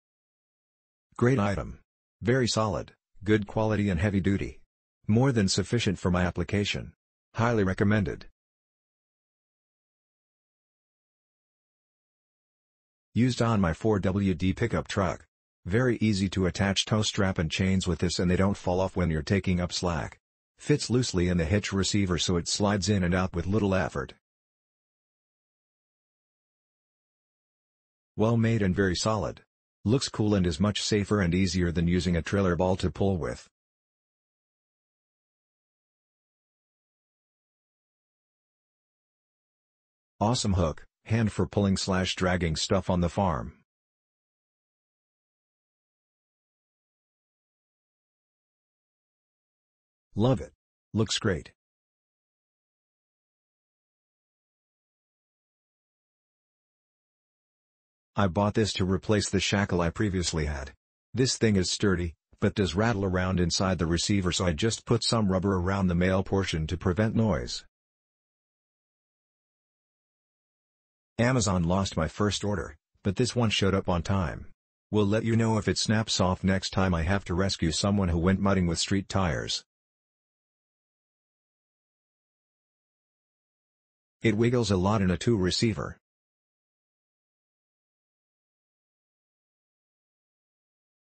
Reese Towpower 7024400 2" Forged Receiver Mount Tow Hook
Great item. Very solid, good quality and heavy duty. More than sufficient for my application. Highly recommended. Used on my 4WD pickup truck. Very easy to attach tow strap and chains with this and they dont fall off when your taking up slack.

Fits loosely in the hitch receiver so it slides in and out with little effort. Well made and very solid. Looks cool and is much safer and easier than using a trailer ball to pull with. awesome hook, hand for pulling/dragging stuff on the farm Love it... Looks great. I bought this to replace the shackle I...